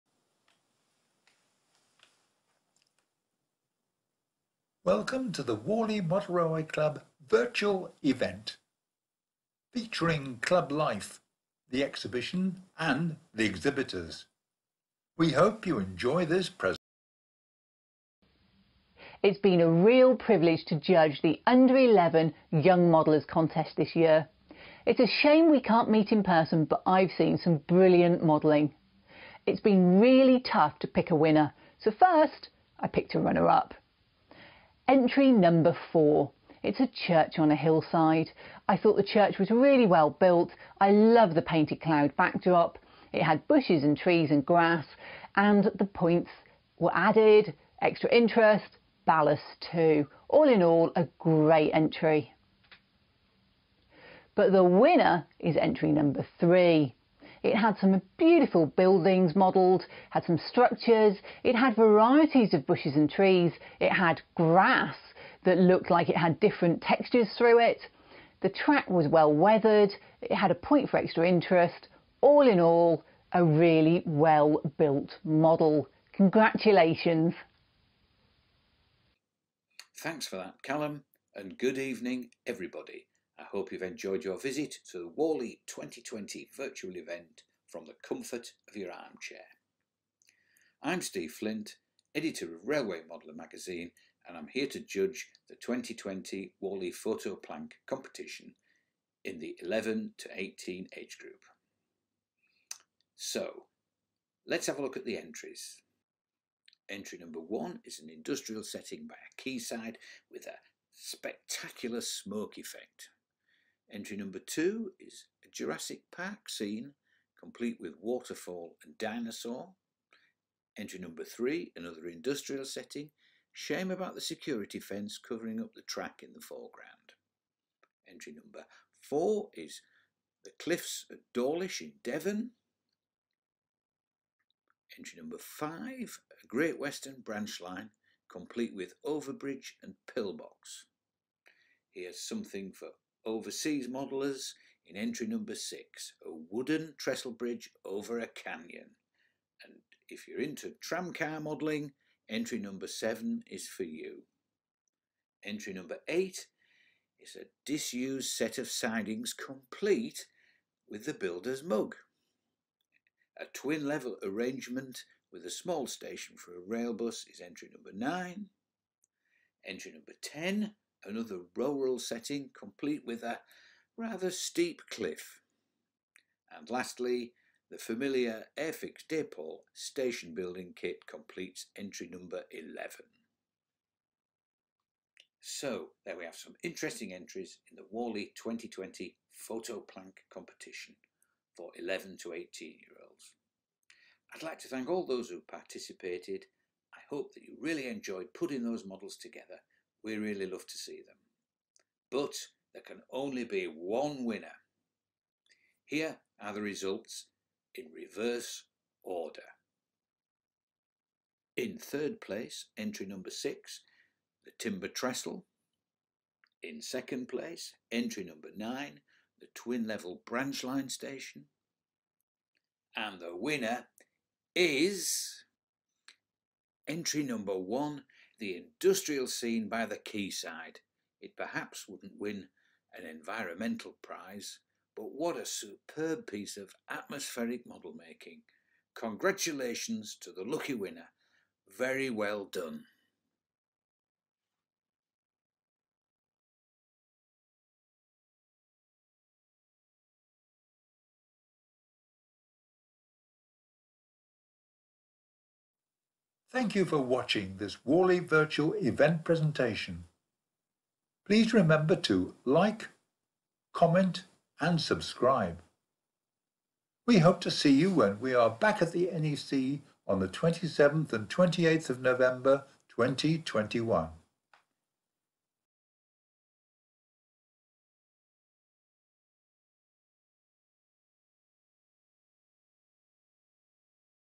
Railway Modeller Photographic Plank Competition Results
And what you have been waiting for all day, the results of the Railway Modeller Junior Photographic Plank Competition, we are joined by Kathy Millat and Steve Flint who judged the competition.

Scale Model Scenery Laser Cut Model Railway Accessories